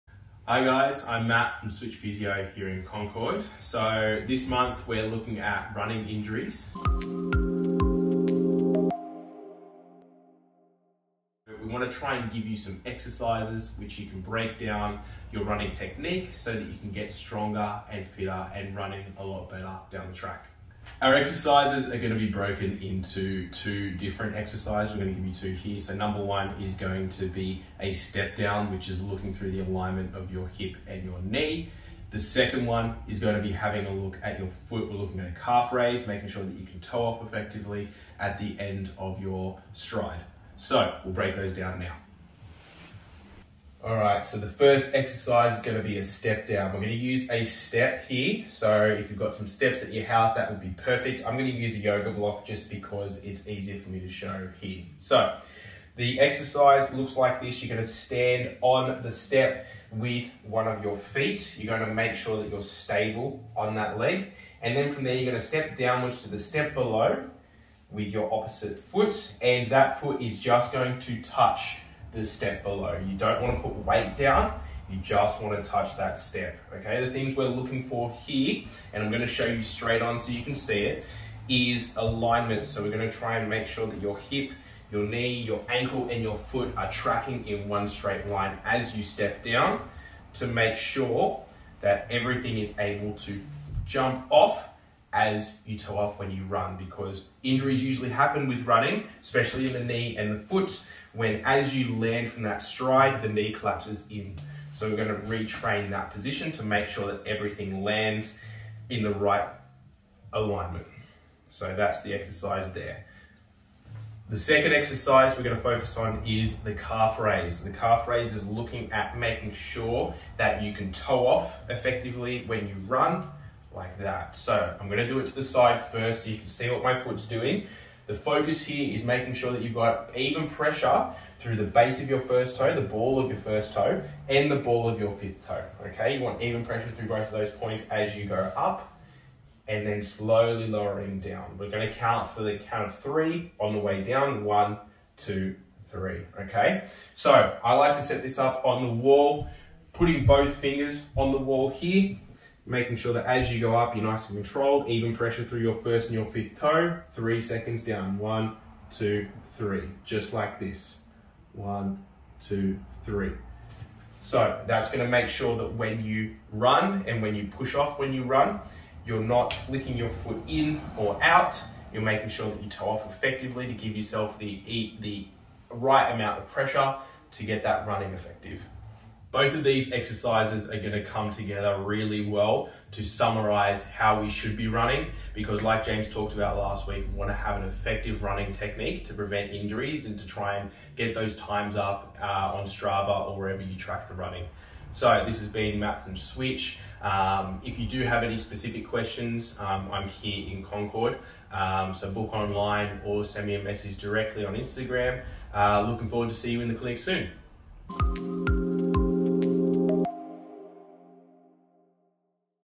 Running Injuries Awareness Month - Exercise Videos
Our physiotherapist, Matt, goes through two basic exercises that focus on the lower limbs and their alignment whilst running. Having the correct alignment and building strength in your legs will limit the long term affects from running on hard ground.